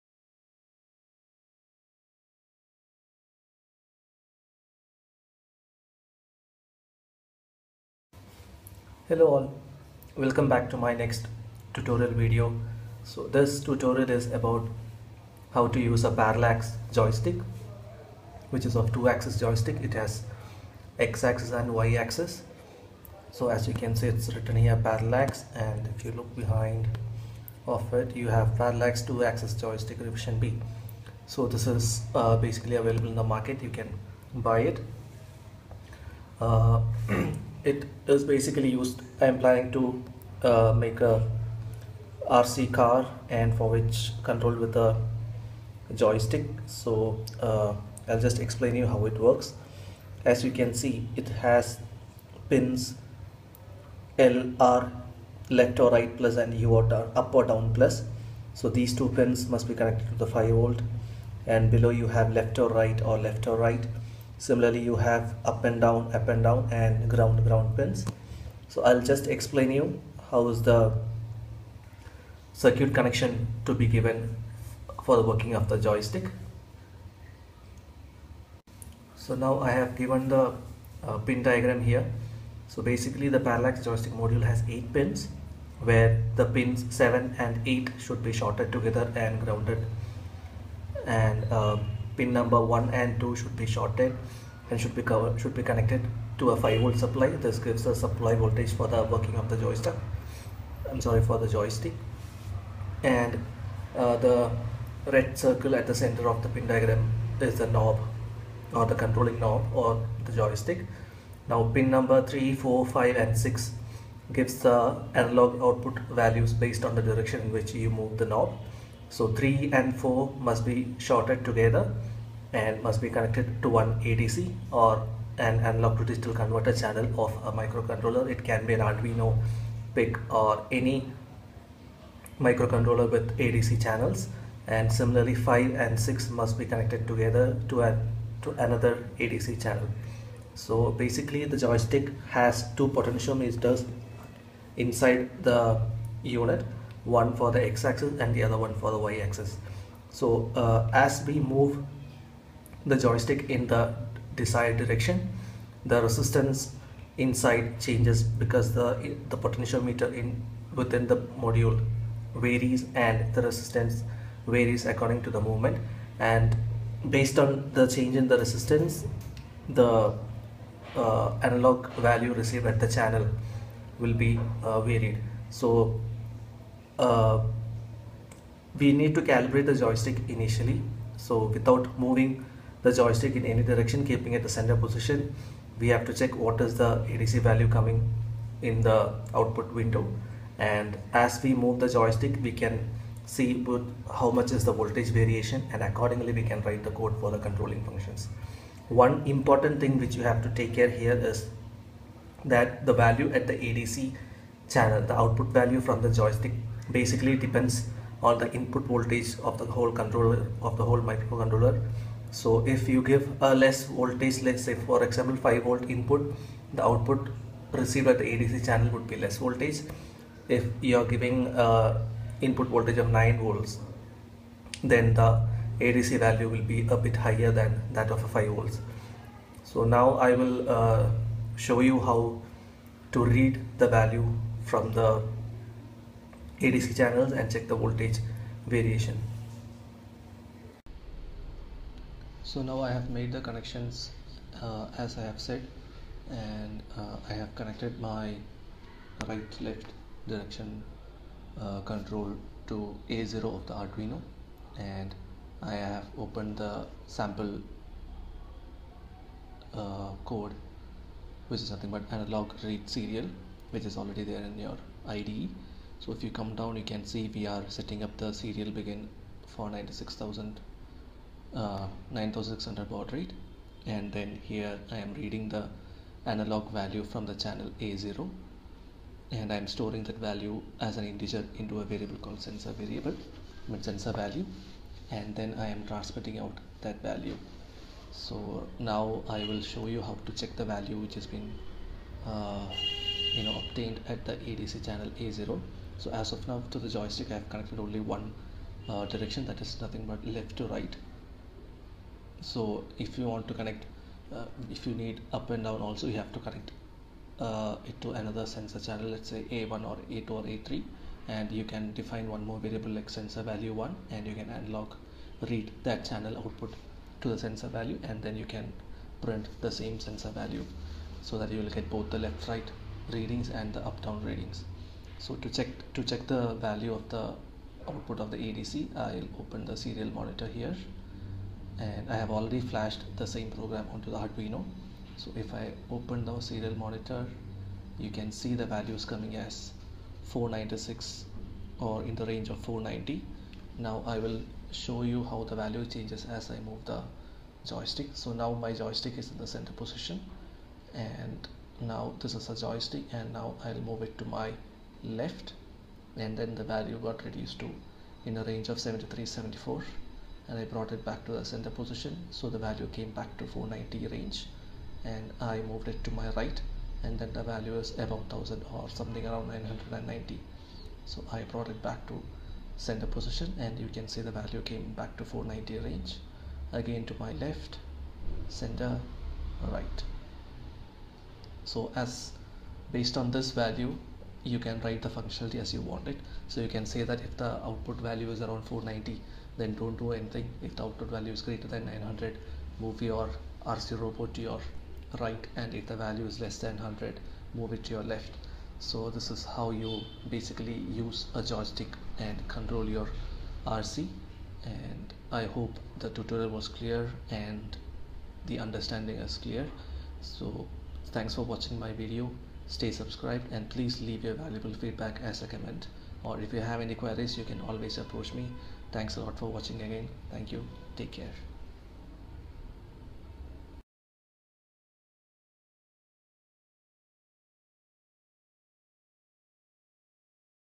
Home made RC Robot : Part I - Joystick Working Tutorial
This is the first part of the tutorial to make a Remote Control Robot at home. This video explains the working of the Joystick and also how to use it to control the Robot Directions.

Please like and leave your valuable feedback as comments

Code and details available in below link :